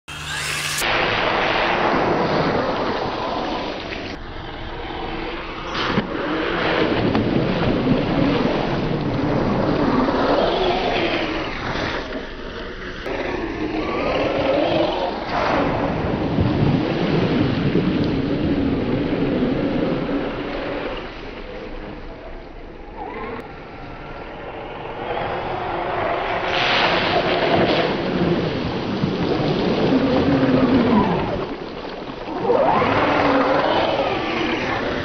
Axial Yeti kit getting wet first run
I just finished building my Yeti kit and this was my first run after getting it all set up. Near the end of the Lipo pack I gave the yeti a bath in this puddle.